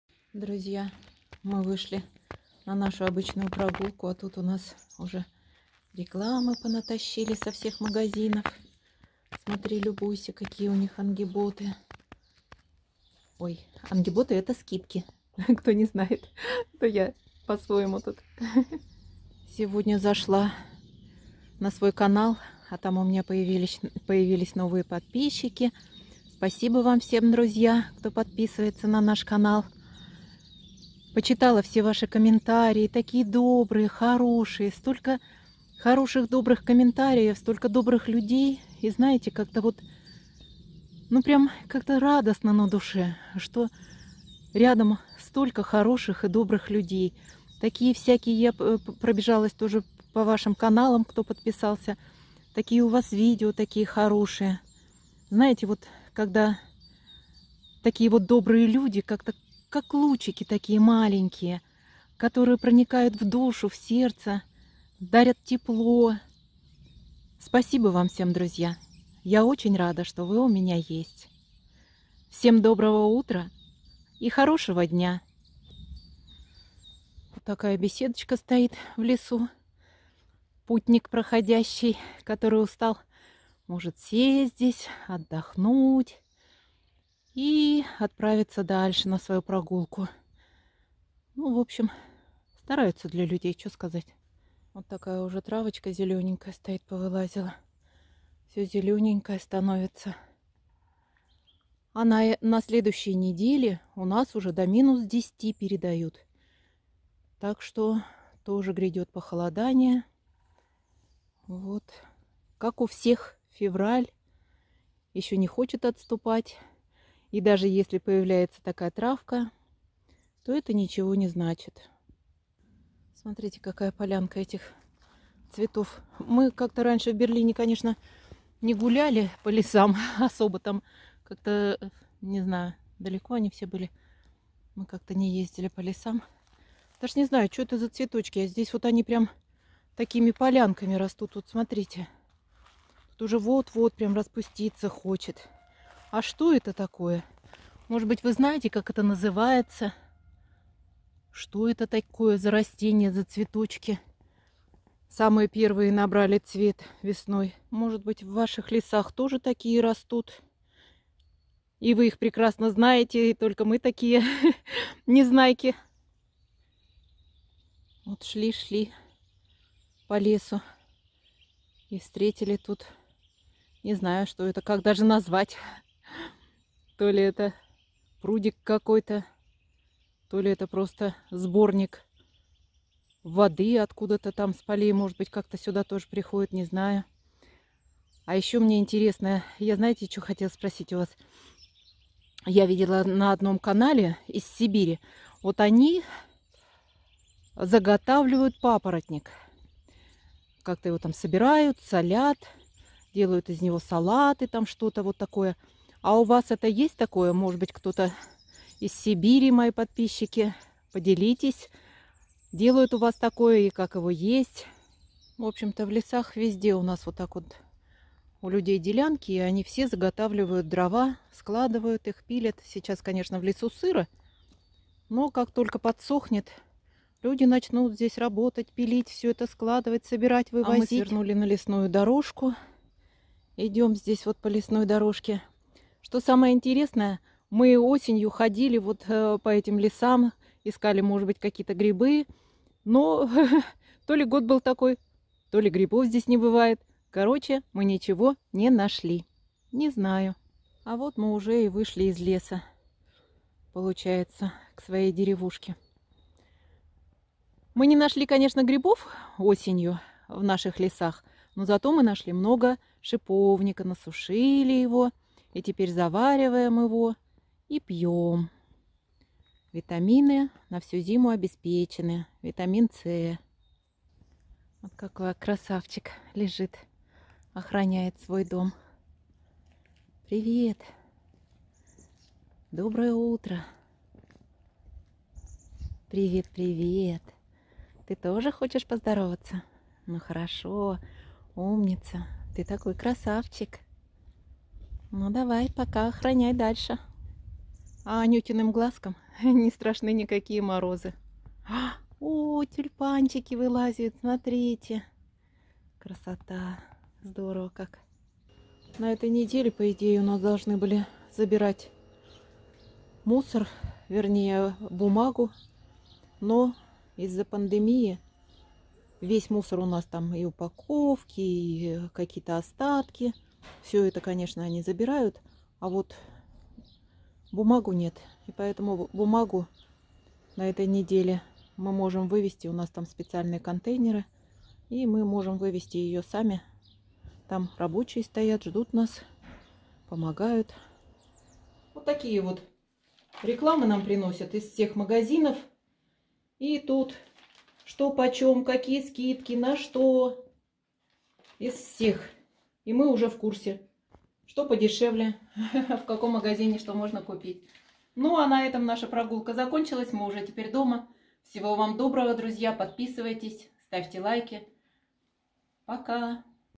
Прогулкав_лесу Беседка Милый_пёсик Тюльпаны_всходят Рекламные_ газеты.
В лесу цветы, беседка лесная, пёс охранник, анютины глазки